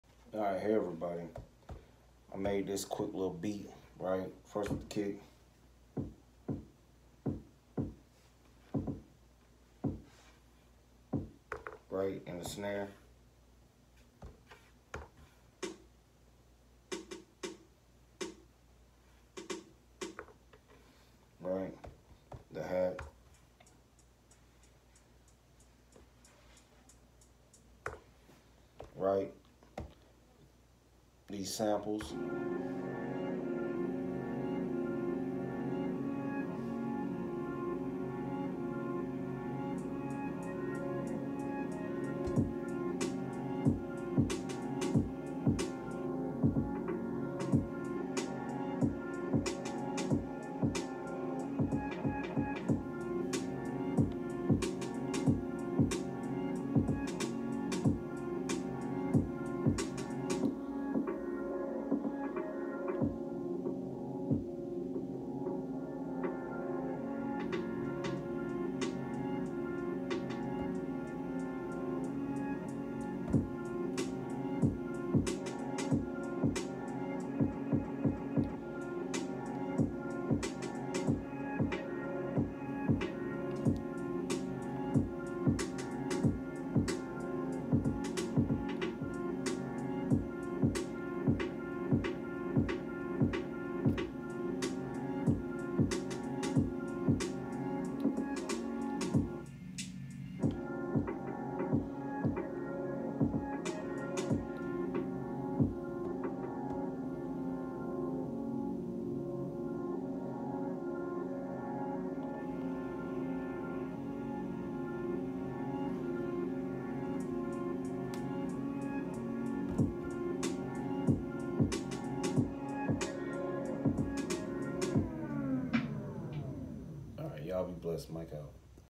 Chillhop Beat on Mpc Live2!chillout
Hey everybody. I’m Mike Venus and I made a simple beat on the Mpc Live2 and perform it. Enjoy ☺️